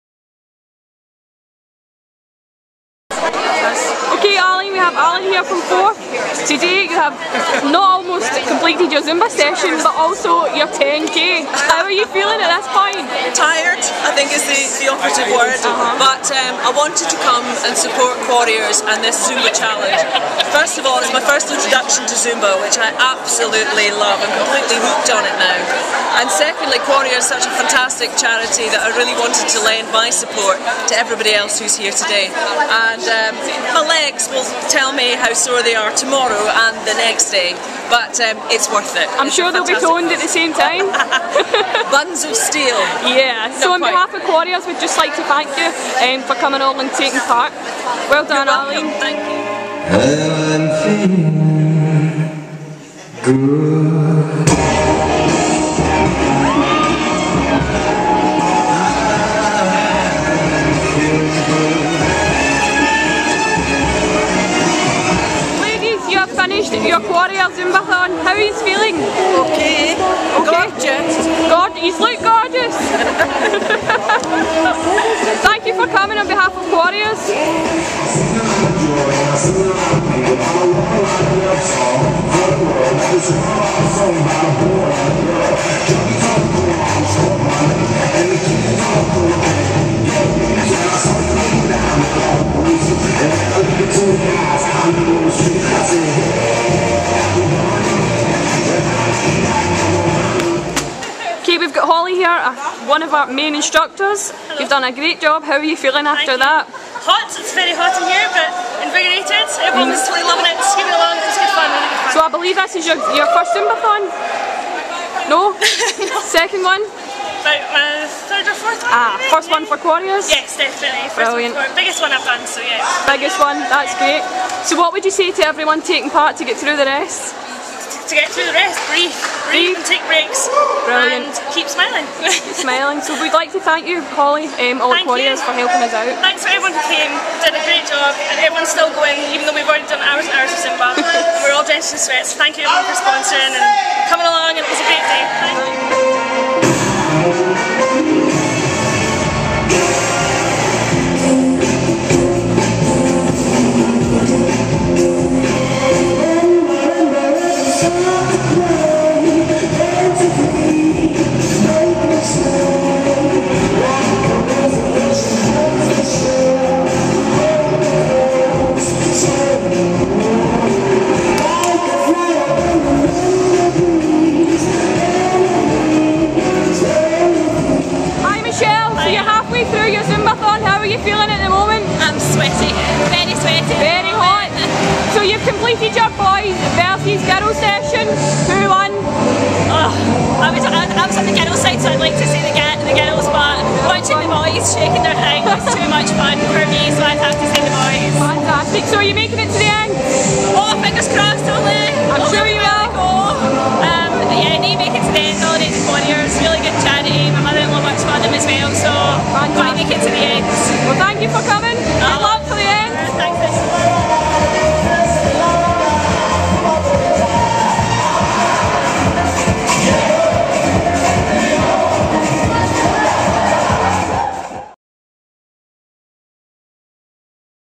The Big Zumbathon Edinburgh 2 October 2011.mp4
Forth One radio presenter tells us how she got on at the Big Zumbathon Edinburgh. If you took part in the event you may spot yourself!